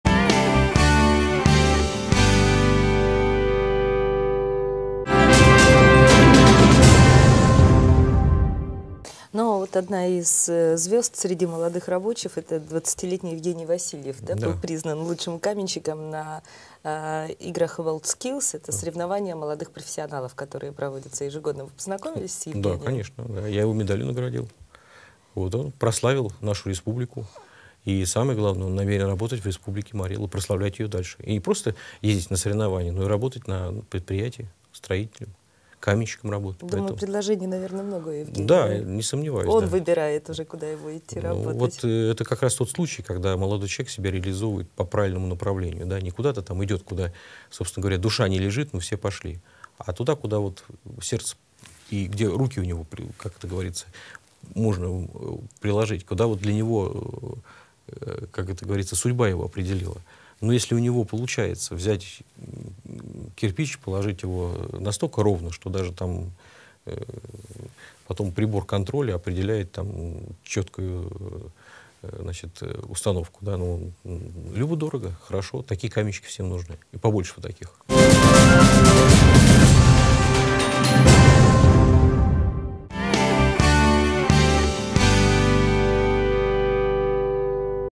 Леонид МАРКЕЛОВ - про участника чемпионата «WorldSkills» Евгения Васильева
Телеканал «РЕГИОН 12». Фрагмент прямого эфира от 07 сентября 2015г.
Интервью с ВРИО Главы Марий Эл Леонидом Маркеловым.